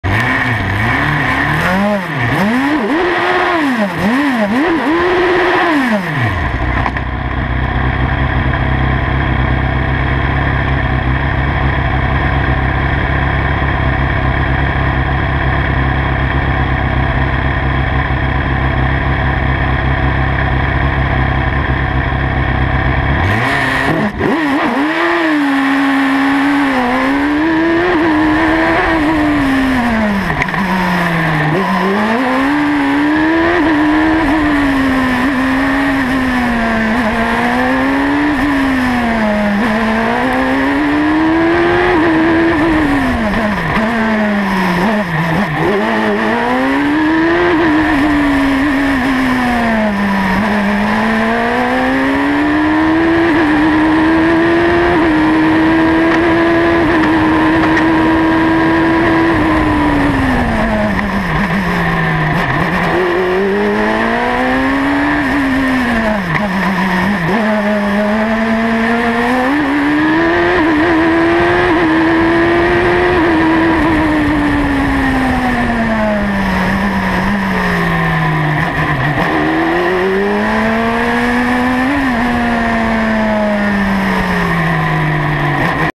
Steve Marr PCD Saxon Loton Park September 2015
Onboard the PCD Saxon at Loton Park for the final BHC rounds of 2015.  My best run of the weekend for 2nd in class (despite the very poor start).